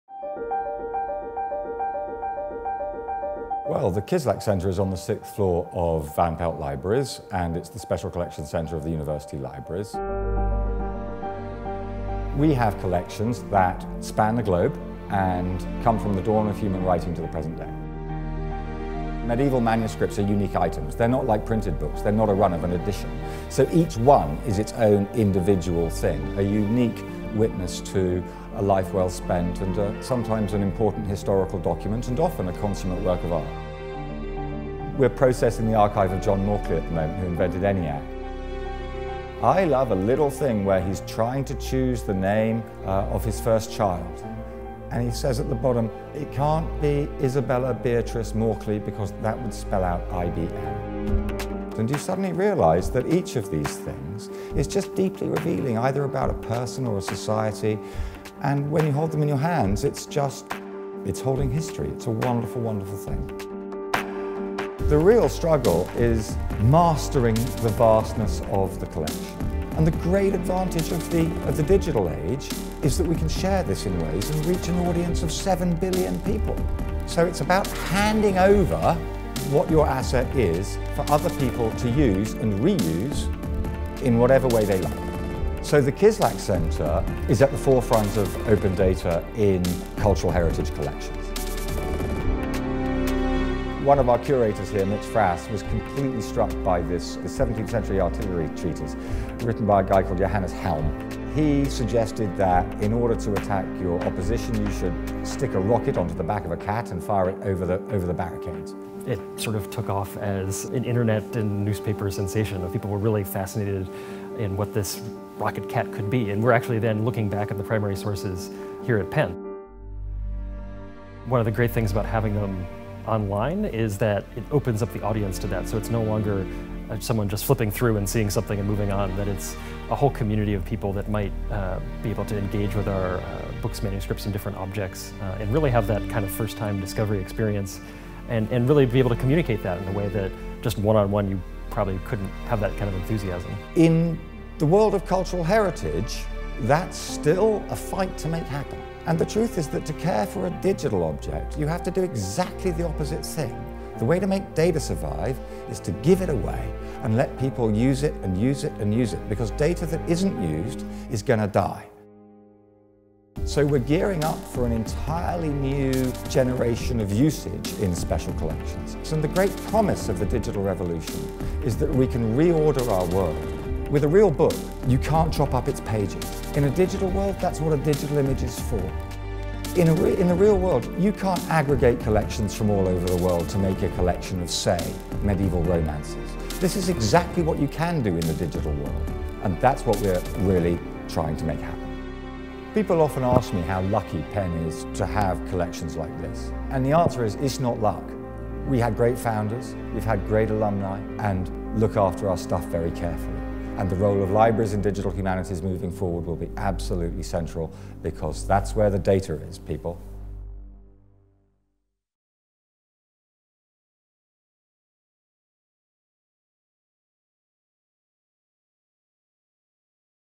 The Kislak Center Embraces Open Data
Will Noel’s goal for Penn’s rare book and manuscript collection is to digitize it and make it as broadly available in as many ways as possible.